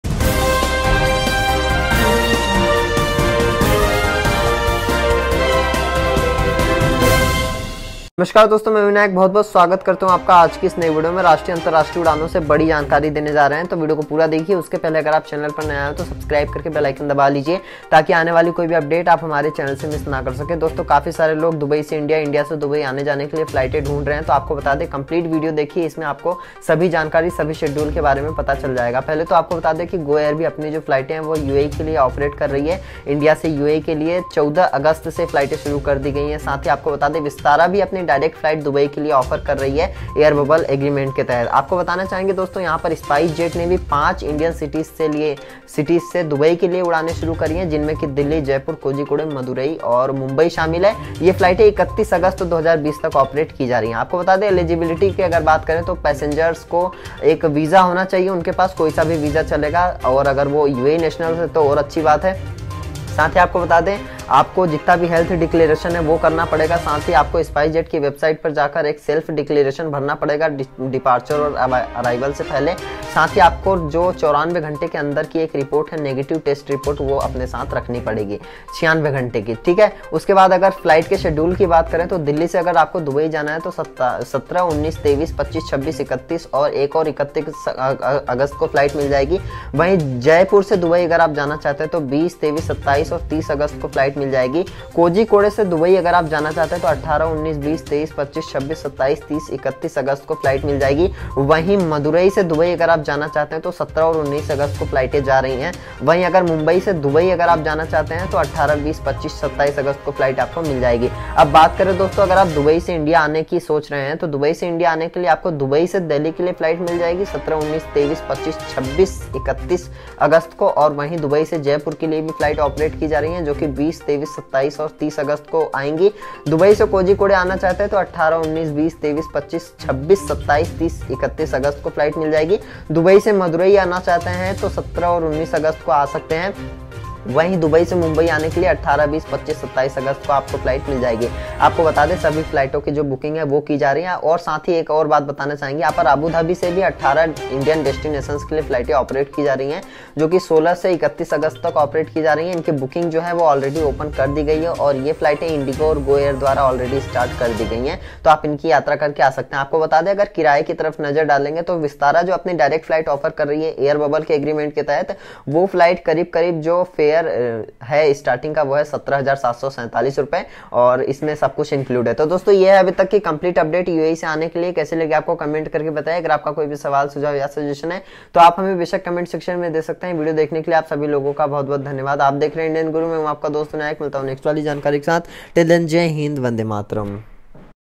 International flights between Dubai and Indian cities to start from tomorrow | New Airlines Resume
Budget carrier SpiceJet will be operating scheduled flights between Dubai and 5 Indian cities – Delhi, Jaipur, Kozhikode, Madurai and Mumbai, from tomorrow to 31st August, 2020. Here is everything you should know about the flights.